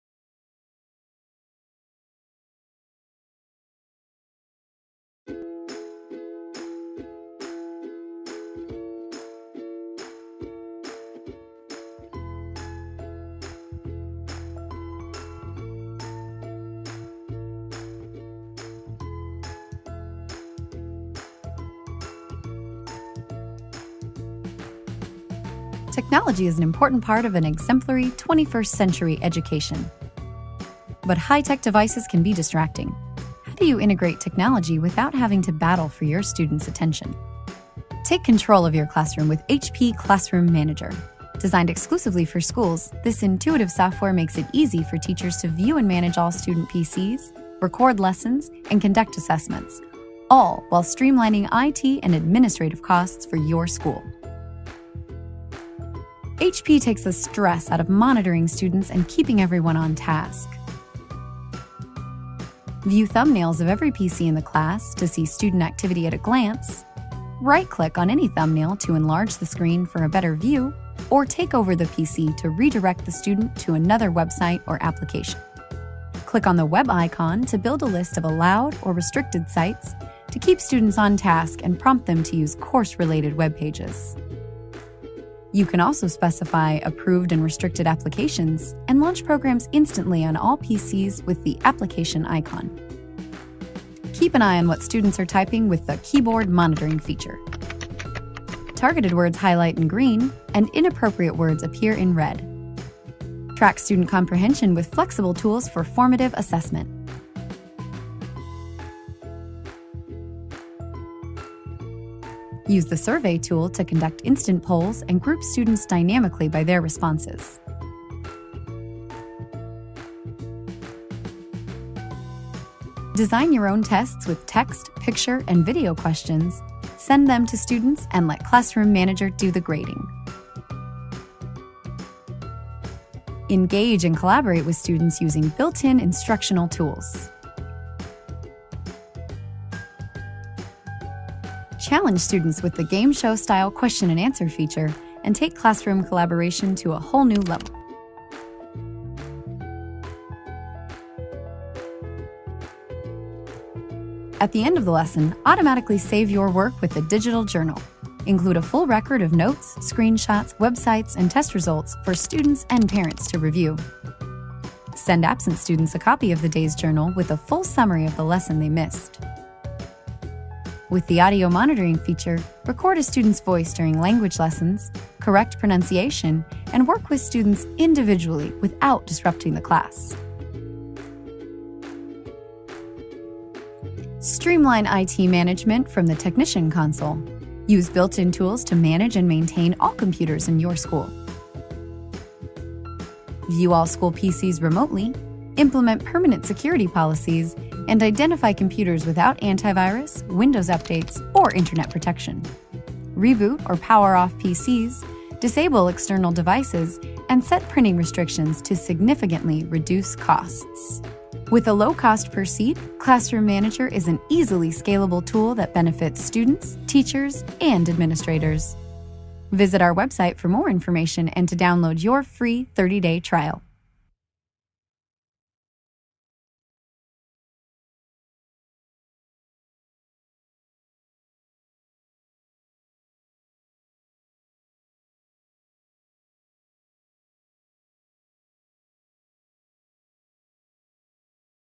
HP Classroom Manager subtitulado
VIDEO DE PRESENTACIÓN HERRAMIENTA DE HP EDUCACIÓN - CLASSROOM MANAGER - PARA LA GESTIÓN EN EL AULA "ONE TO ONE".